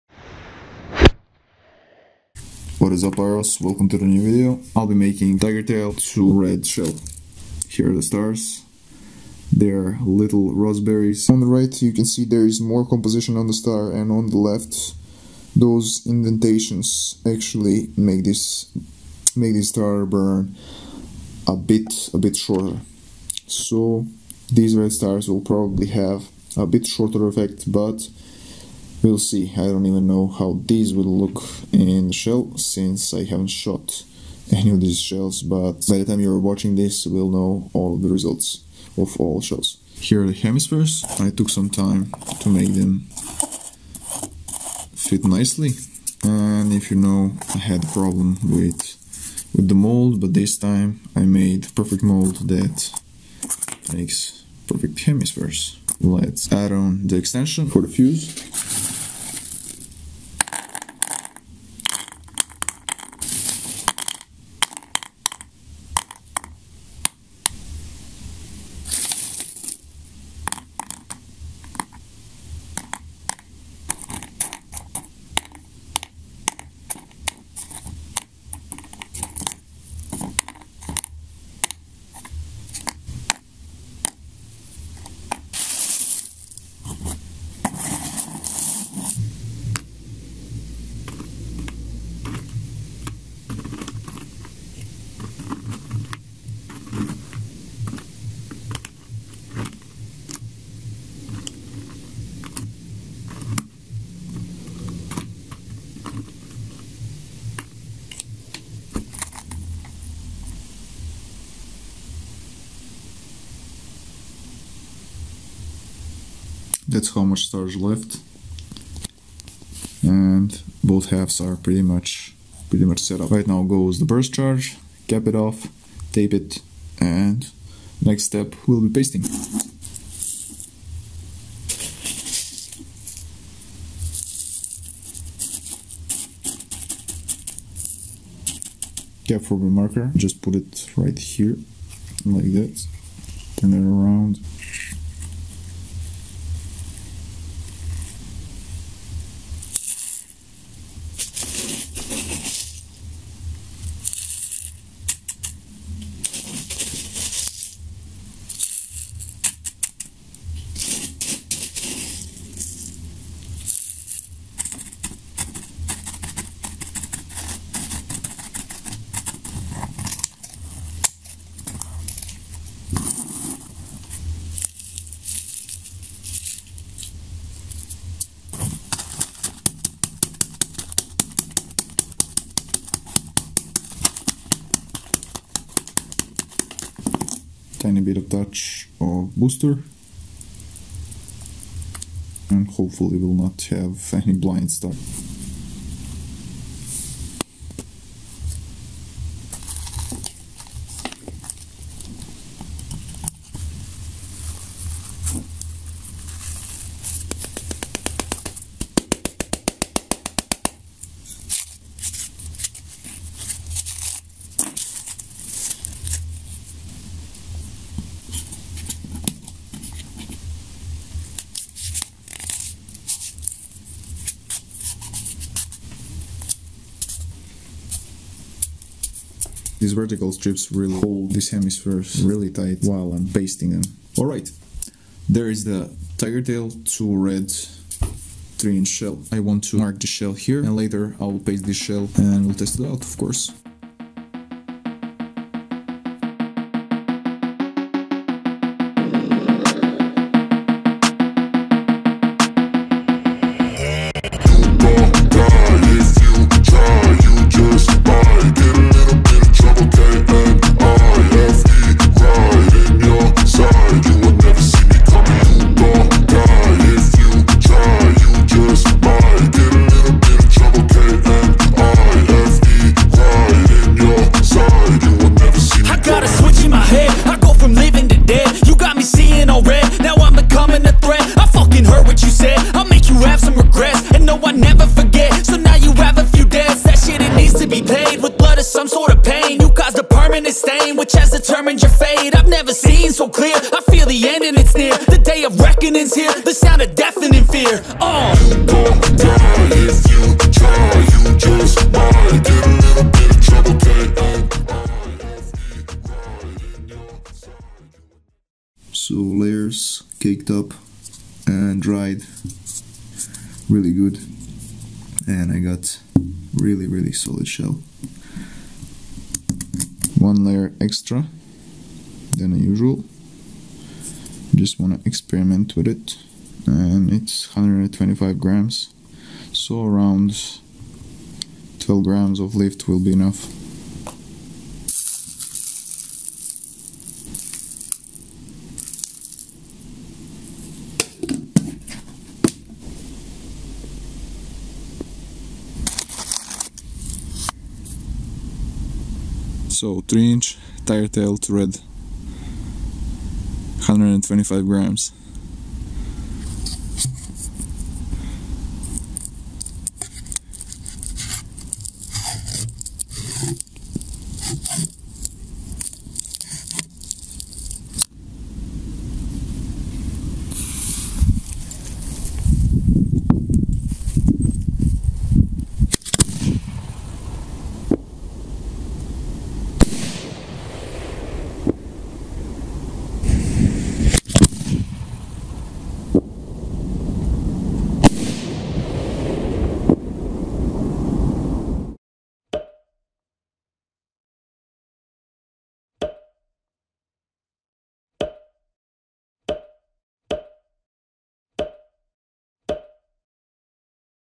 Making & Shooting Gorgeous 3 INCH Firework Shell [Willow to Red]
Making firework shells is a journey of its own, it's an art, combine that with filming all of that and you get plenty of work on your plate haha! It can get exhausting but in the end, you become proud of the work you've done.
Also, fast flash was used to boost this shell.